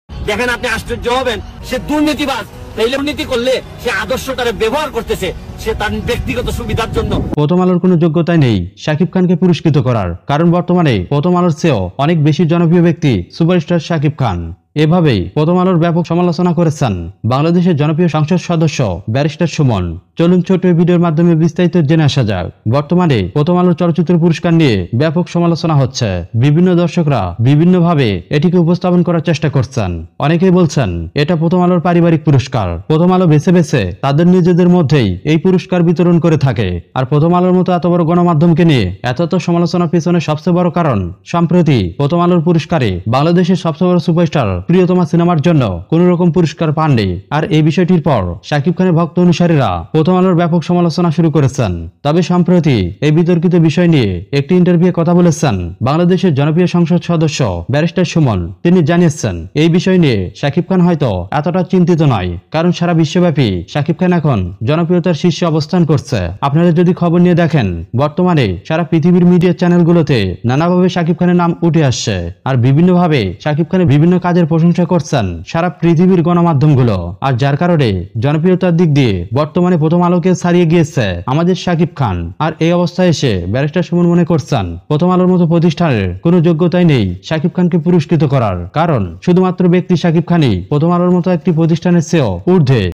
শাকিব খানের পক্ষ নিয়ে প্রথম আলোকে হুংকার দিলেন ব্যরিস্টার সুমন। Shakib Khan & Barrister Sumon।
শাকিব খানের পক্ষ নিয়ে প্রথম আলোকে হুংকার দিলেন ব্যরিস্টার সুমন। Shakib Khan & Barrister Sumon। শাকিব খানের পক্ষে কথা বললেন এম পি ব্যরিস্টার সুমন। শাকিব খান কে নিয়ে বক্তব্য রাখলেন এম পি ব্যরিস্টার সুমন।